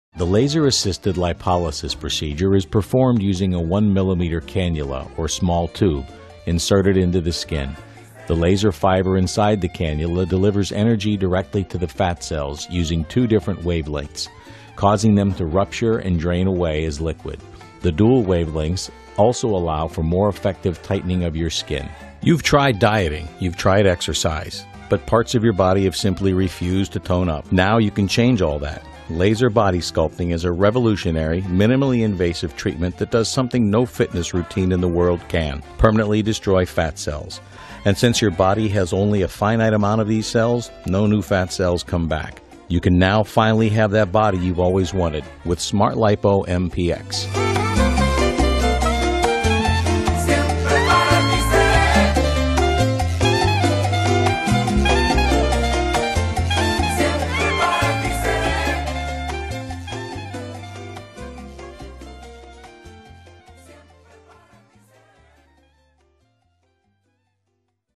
6. Laser Assisted Lipolysis Procedure - Liposuction (SmartLipo)
- Learn how a Laser Assisted Lipolysis Procedure or Liposuction is performed.